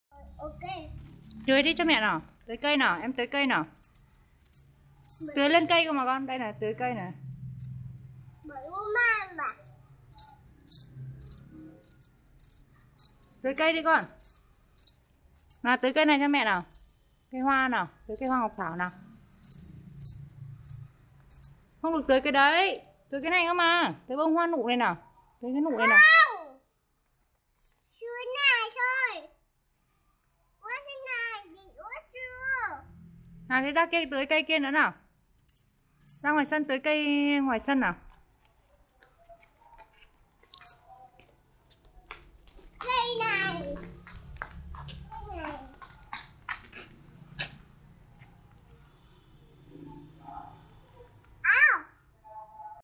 Bé Tú Anh tưới cây cho mẹ
Bé 2 tuổi tưới cây giúp mẹ